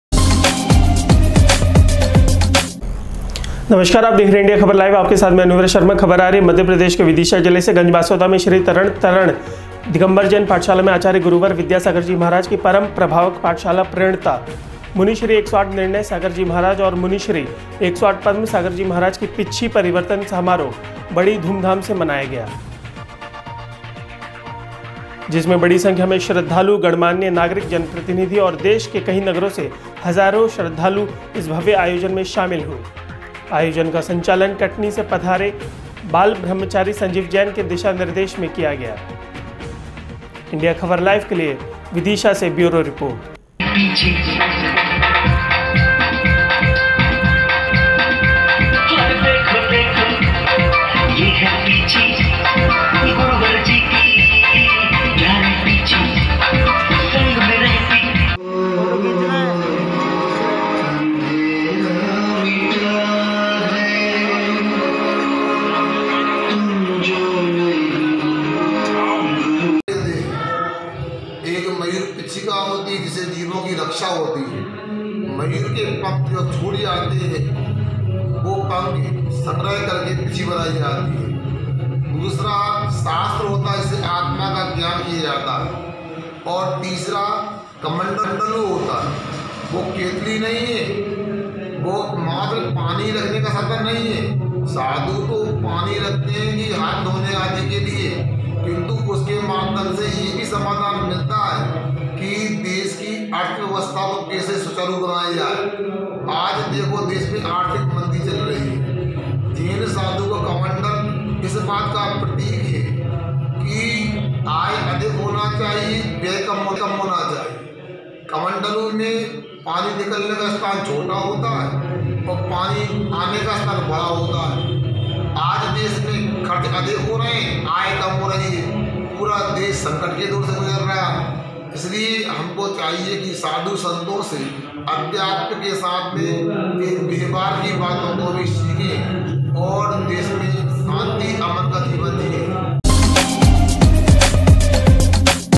गंजबासौदा || पिच्छी परिवर्तन समारोह, हर्षोल्लास के साथ मनाया गया || इंडिया ख़बर LIVE
पिच्छी परिवर्तन समारोह, हर्षोल्लास के साथ मनाया गया

गंजबासौदा में श्री तारण तरण दिगम्बर जैन पाठशाला में आयोजन हुआ

मुनि श्री 108 निर्णय सागर जी महाराज और मुनि श्री 108 पद्म सागर जी महाराज का, पिच्छी परिवर्तन समारोह मनाया गया

बड़ी संख्या में भक्त, गणमान्य नागरिक, जनप्रतिनिधि शामिल हुए

आयोजन का संचालन कटनी से पधारे, बाल ब्रह्मचारी संजीव जैन ने किया